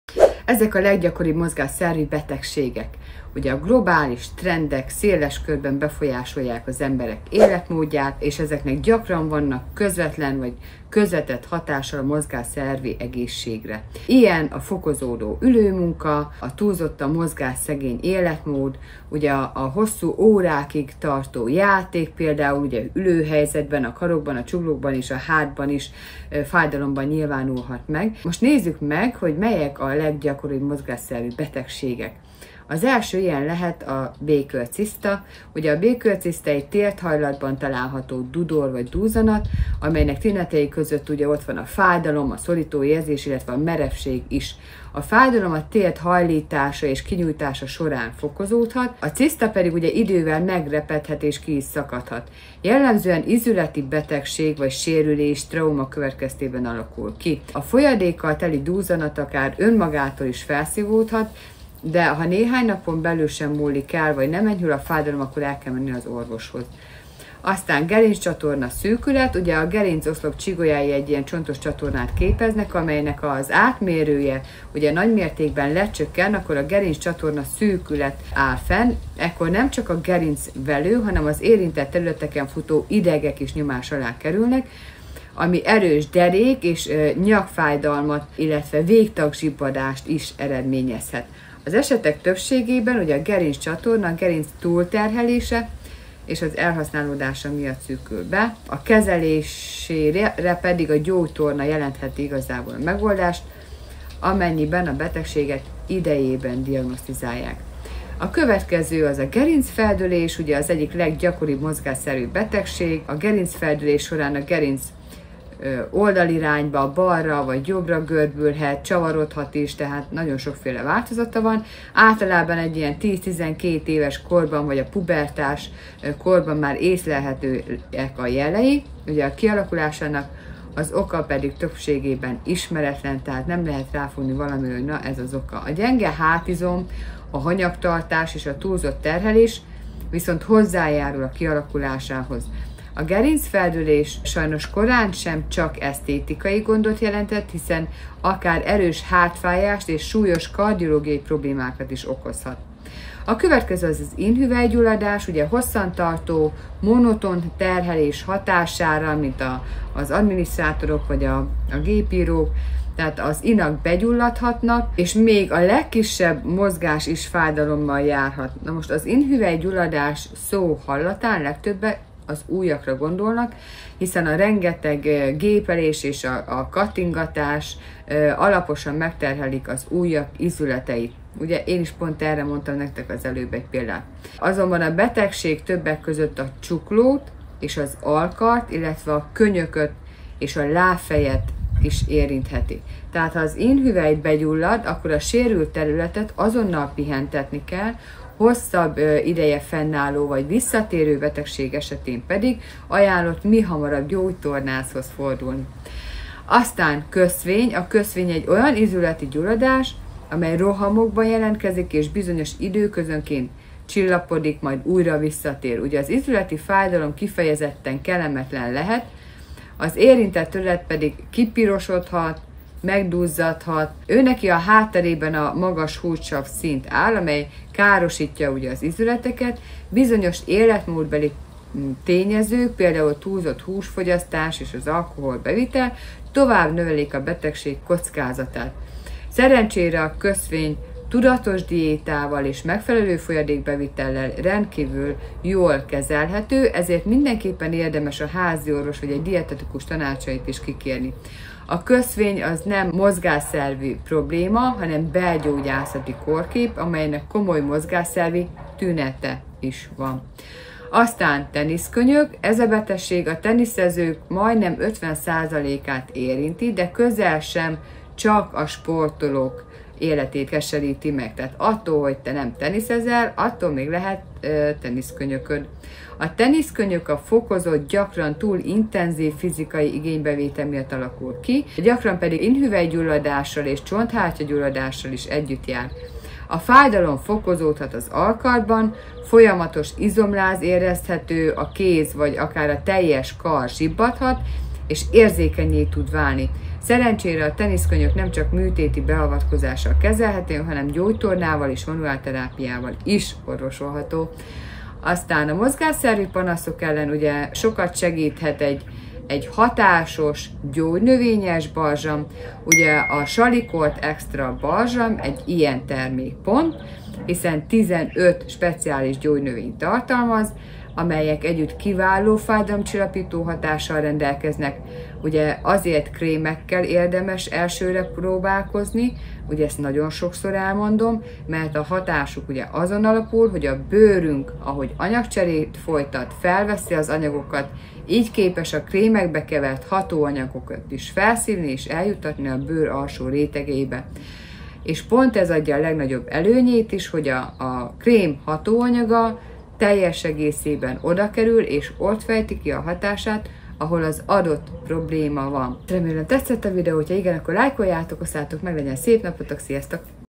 Ezek a leggyakoribb mozgásszervi betegségek! ☝️ - hillvital.hu
A globális trendek széles körben befolyásolják az emberek életmódját, és ezeknek gyakran vannak közvetlen és közvetett hatásai a mozgásszervi egészségre. Ilyen a fokozódó ülőmunka és túlzottan mozgásszegény életmód. Most nézzük meg, hogy melyek a leggyakoribb mozgásszervi betegségek.